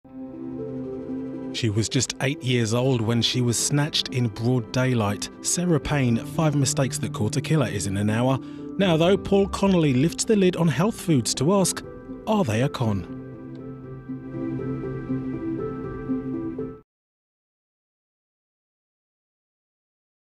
Channel 5 ident 2020 - Dark Green B
One of two 'dark green' variants of the Channel 5 ident from the new look launched on 26 February 2020.